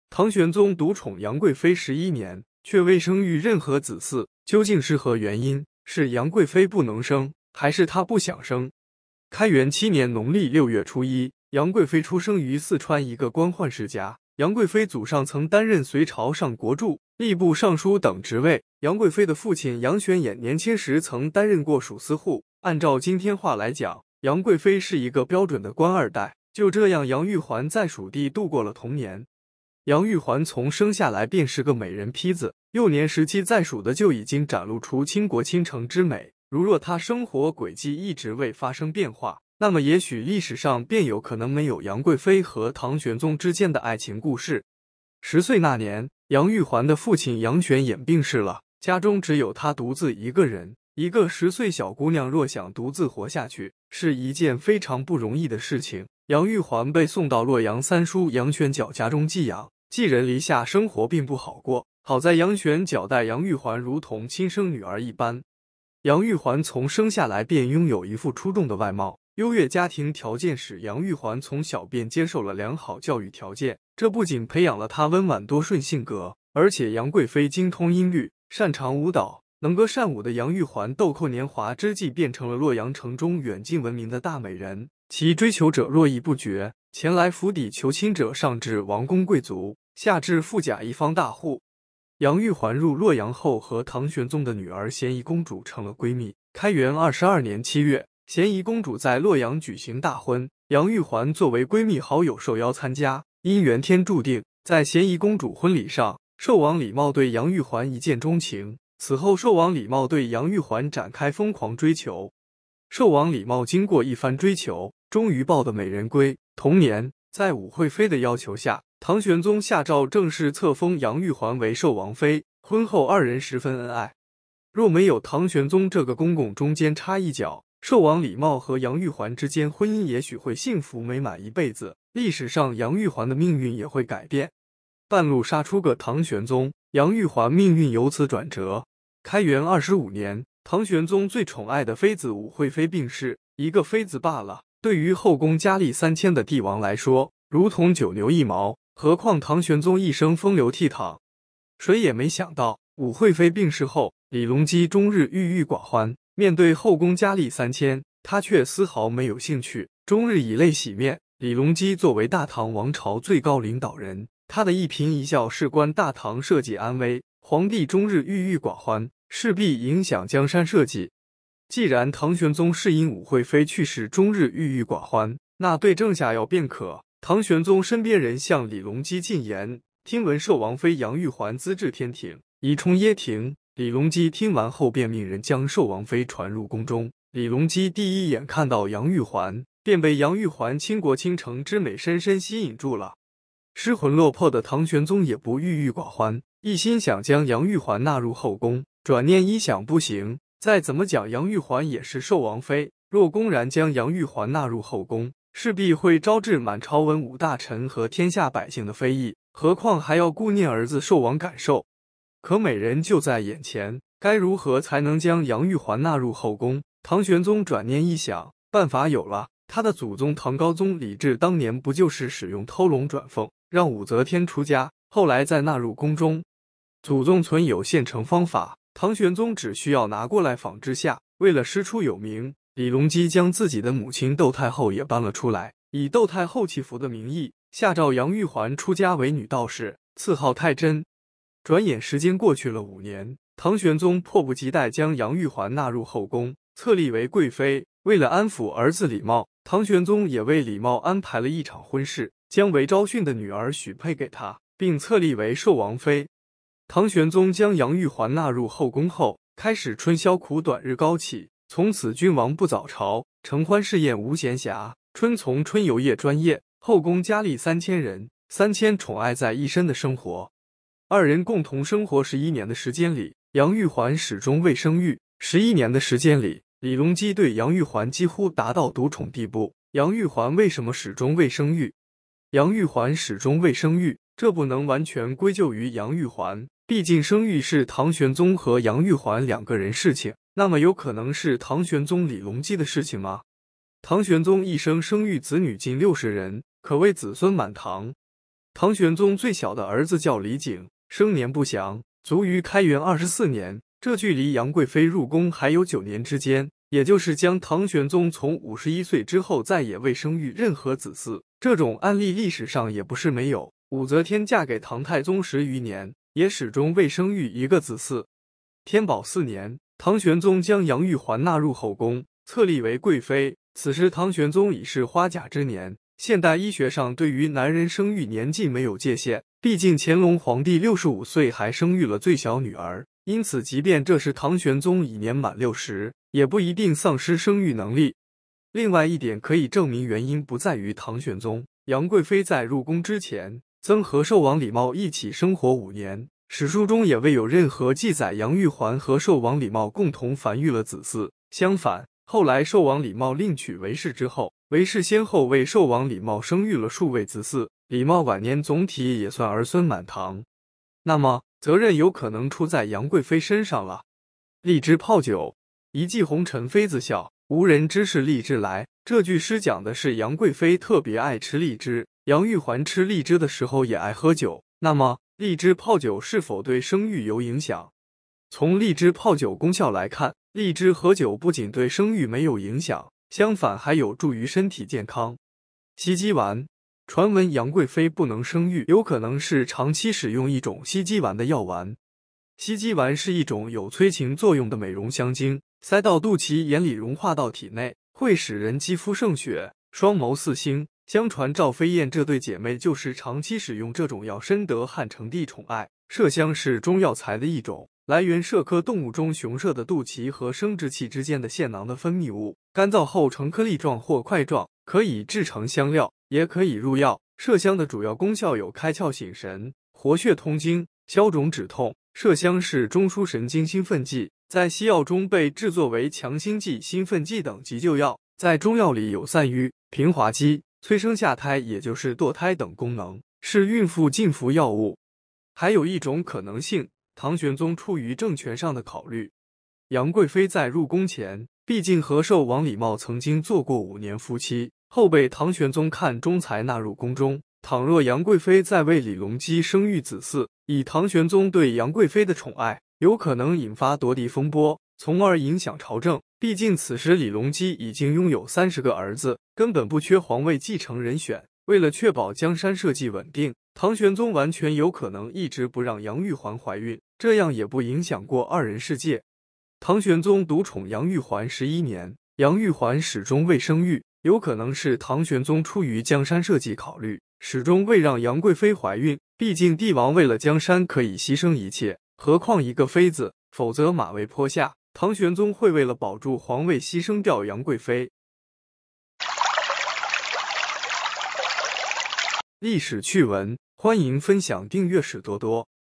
杨贵妃得到了唐玄宗的专宠，为什么膝下没有一儿半女呢？杨贵妃
唐玄宗独宠杨贵妃十一年，却未生育任何子嗣，究竟是何原因?是杨贵妃不能生，还是她不想生?